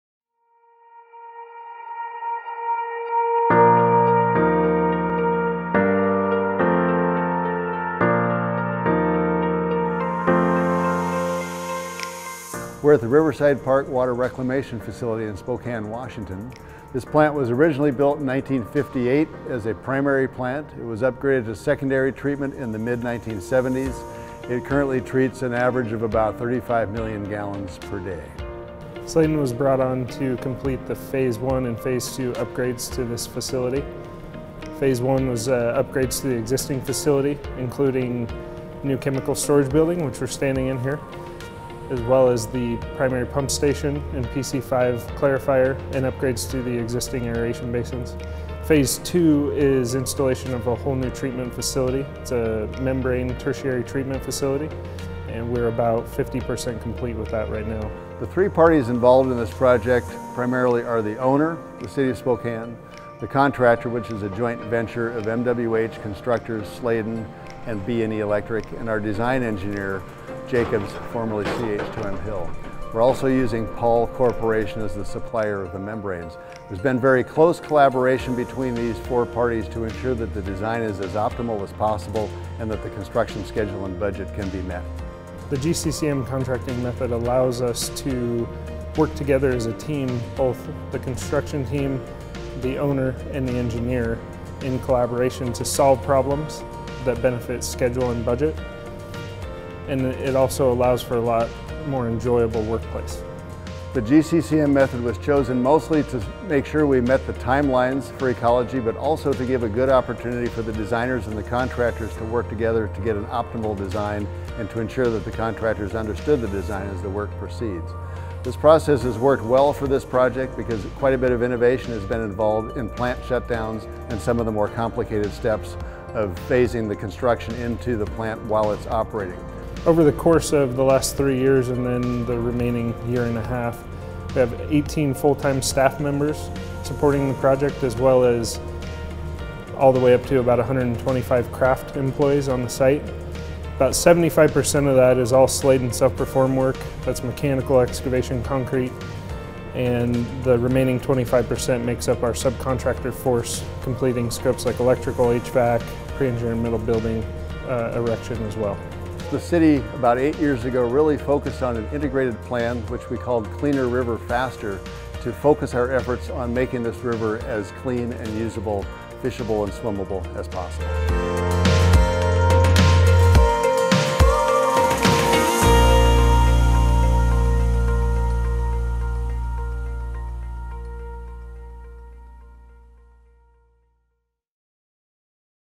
Riverside Park Water Reclamation Facility Next Level Treatment
Spokane, WA
Delivery Method: CMAR
Completion: 2020

This project is a part of the Spokane’s Cleaner River Faster initiative. The Riverside Facility treats approximately 34 mgd of wastewater, capable of treating a peak flow of 150 mgd. The project includes a new chemical facility, new primary clarifiers, a new primary solids pump station and modifications to the headworks, pumping facility and tunnels.